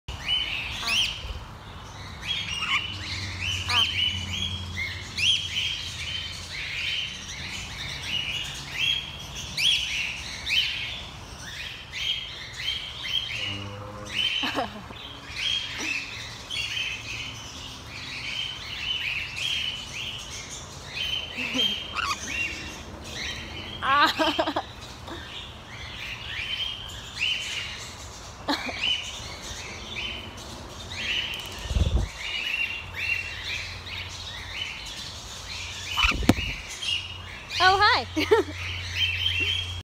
Checkers, our self-appointed dental hygienist, shows us the importance of proper dental hygiene!
A little birdie told us it's National Dental Hygiene Month.

Tag your favorite dental hygienists, and thank them for keeping our mouths clean!

Doccupations thanks Checkers, our self-appointed dental hygienist, for showing us the importance of proper dental hygiene!